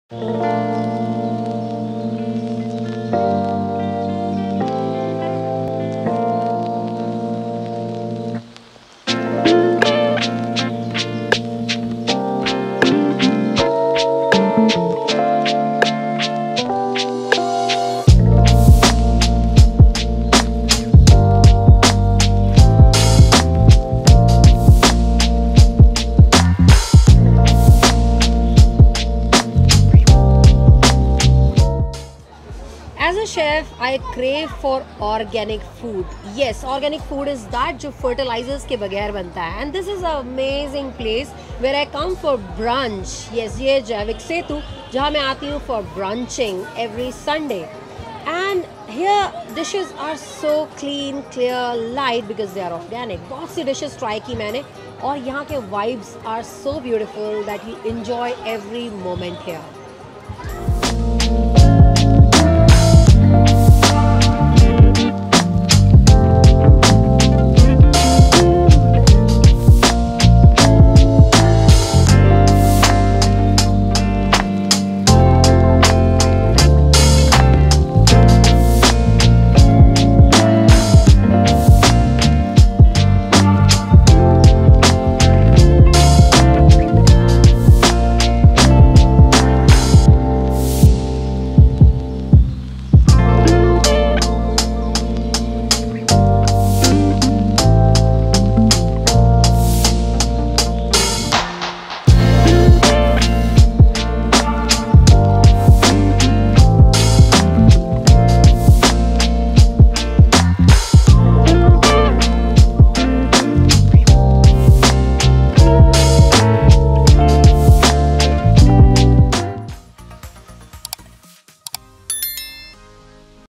Jaivik Setu - We are Eco Friendly | Organic Food Really Healthy - जैविक सेतु | Chef Lata Tondon
Organic food refers to food products that are produced, prepared, and processed without the use of any chemicals. It means organic food production prohibits the use of chemical pesticides, chemical fertilizers, or chemical preservatives. Consequently, organic foods are increasingly gaining popularity because a greater portion of the population wants to know its benefits. The general public belief is that organic food is healthier compared to the conventional ones, and it’s the primary reason for its increased demand over the past decade.

Jaivik Setu (जैविक सेतु) comes alive every Sunday morning with its immensely popular farmers' market and cafeteria serving scrumptious organic fare. This is the place to be to pamper your taste buds, sit back and relax on Sundays or to enjoy special events including popup stalls from chefs, live music, workshops, and more. Shoppers can indulge in grocery and handicrafts shopping throughout the week.